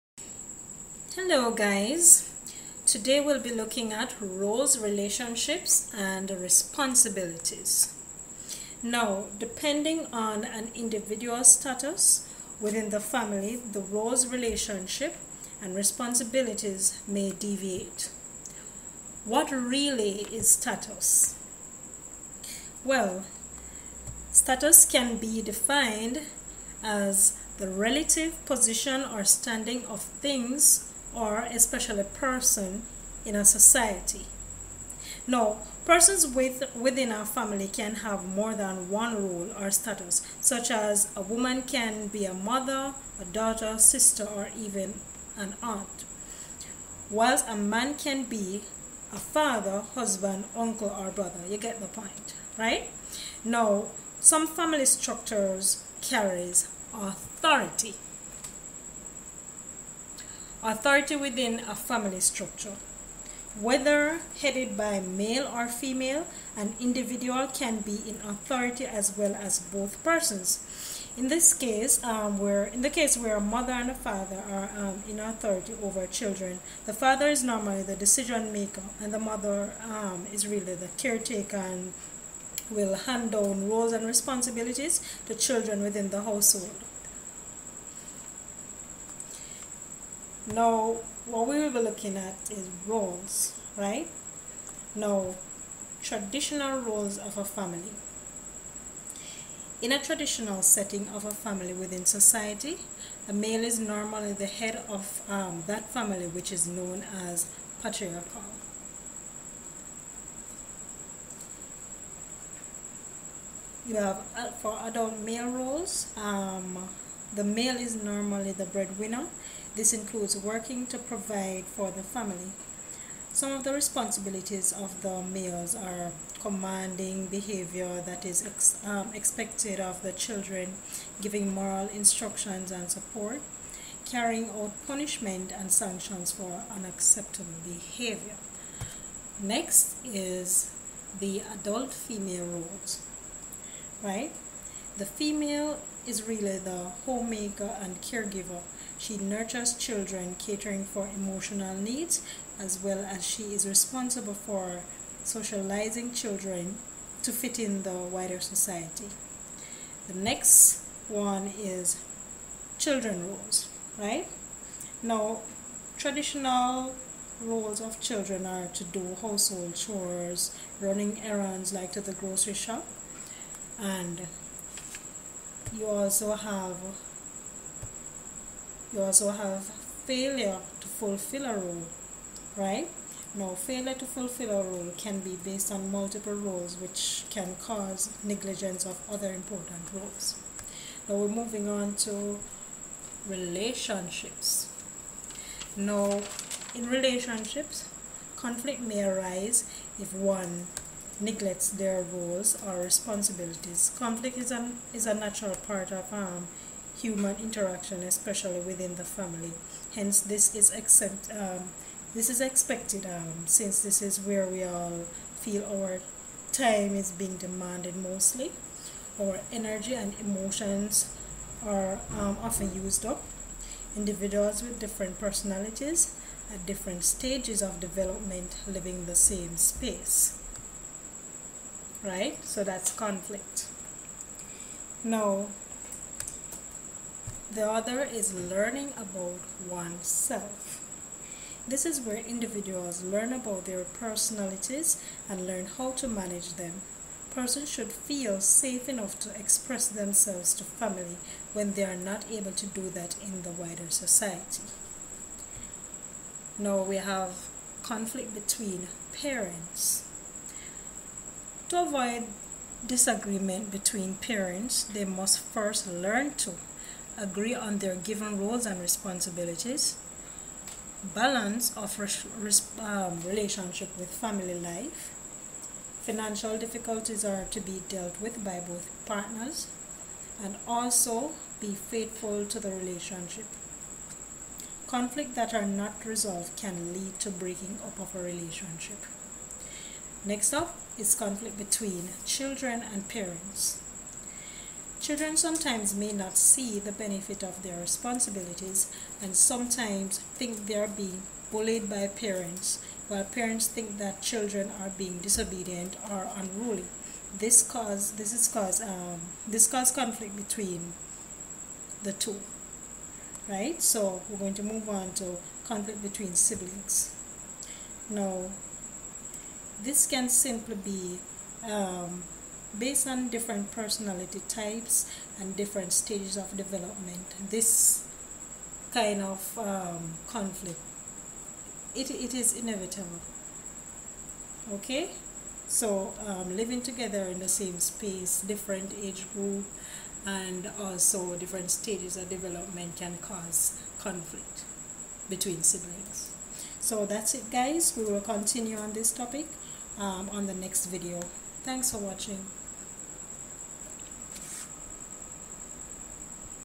Roles,Relationships and Responsibilities(CSEC Social Studies Lecture Series)
This is a video explaining the Roles, Relationships and Responsibilities of a family in details. This video was made mainly for the purpose of students preparing for their CSEC exams.